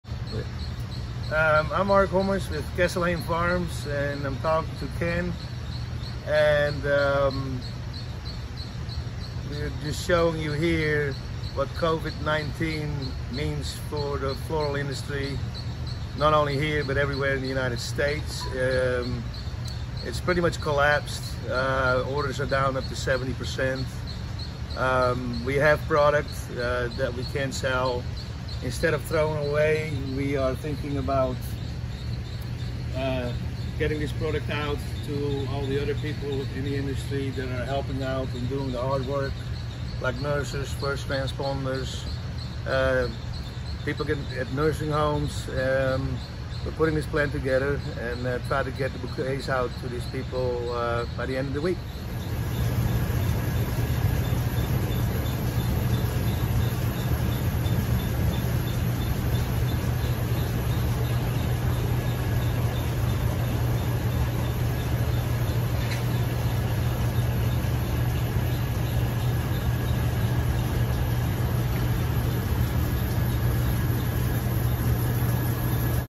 Mark Hommes Castle Hayne Farms
Mark Hommes talks about the affects COVID-19 has had on Castle Hayne Farms.